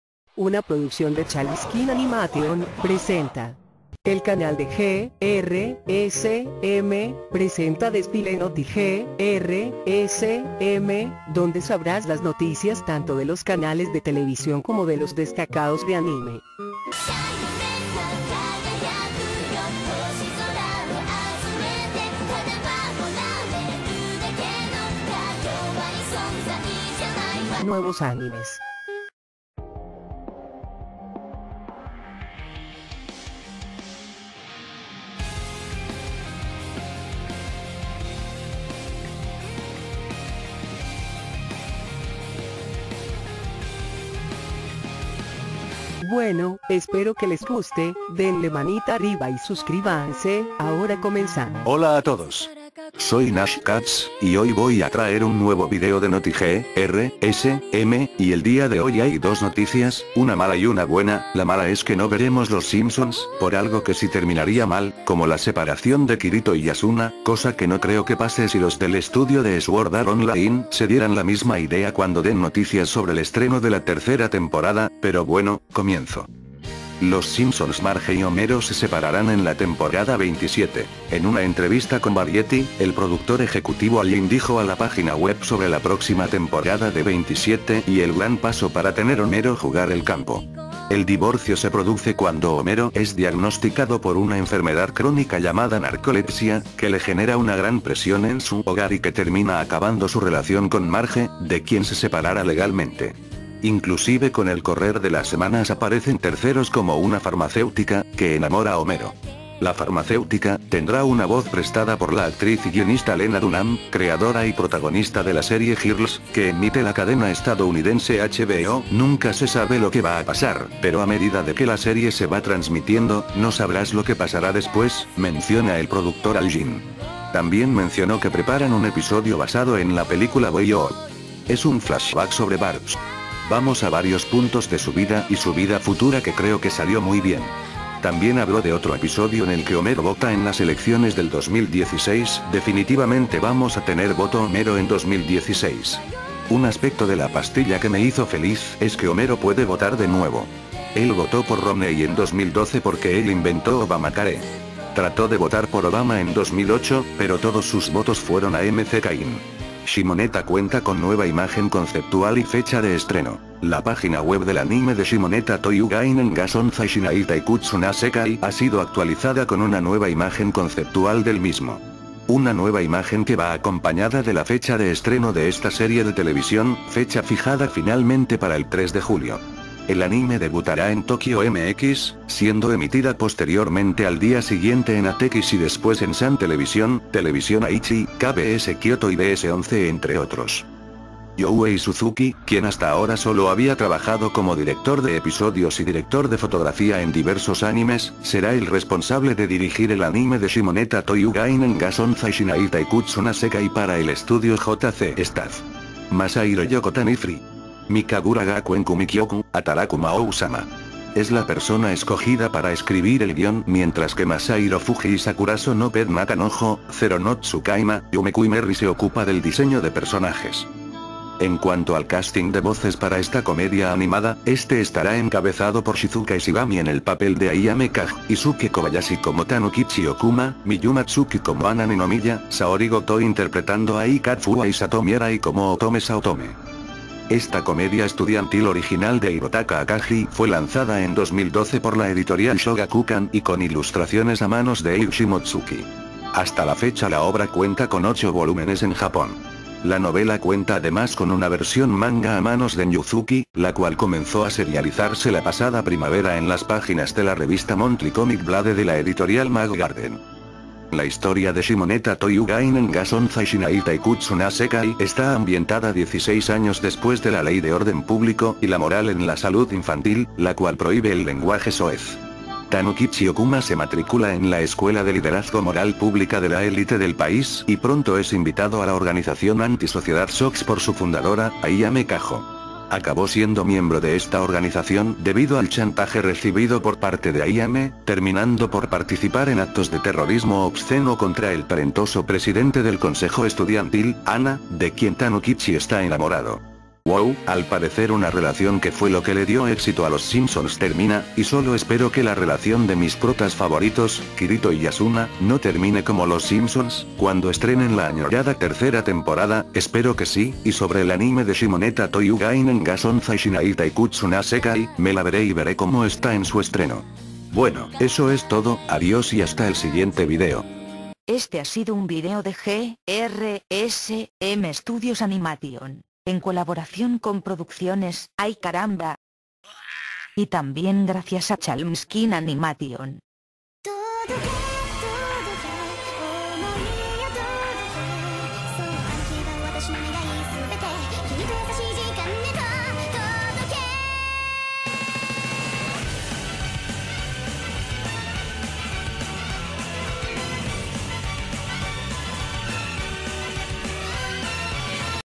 NOTI G,R,S,M SEASON 27 THE SIMPSONS HIMER Y MARGE SE SEPARAN Y EL ANIME DE shimone
Bueno en USA, al parecer en la nueva temporada de los simpsons se dio una posibilidad que la pareja que nunca se a separado, homero y marge,se serpararan, legalmente, es como si kirito y asuna se quieran separar, pero espero que esto no arme el escandalo, como la muerte de brian en family guy.
En japon el anime de shimone, se estrenara en julio, espero que el anime sea bueno.